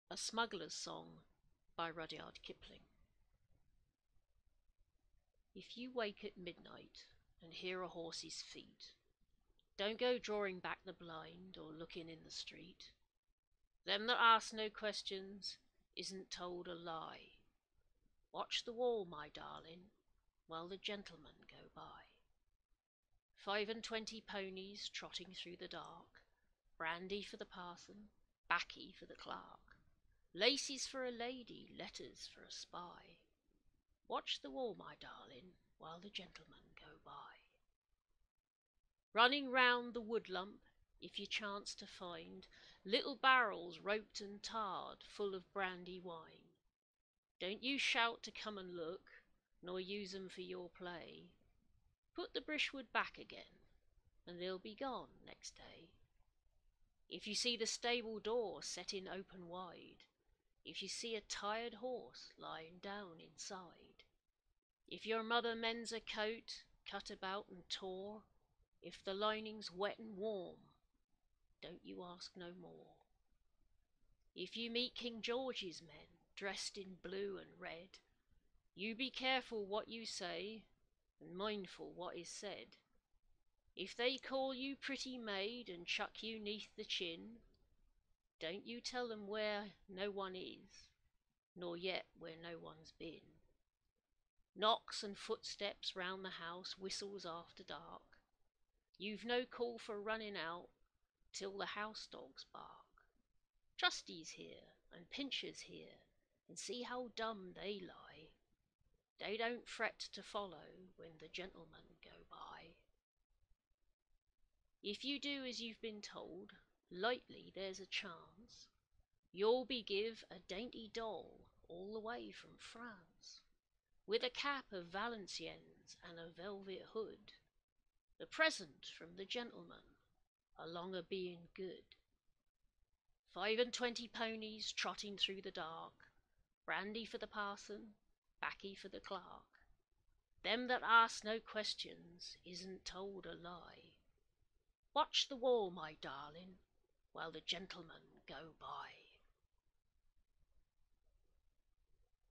Day 69 - The Smugglers Song by Rudyard Kipling (1865 - 1936)
Joseph Rudyard kipling was an English journalist, short story writer and novelist. Born in India it was a significant influence on his work . A prolific writer ,his stories include " The Jungle Book"  and the "Just So" stories; his poems include " If" and  "Mandalay".
Artwork:RRubicante
Voice: Jenois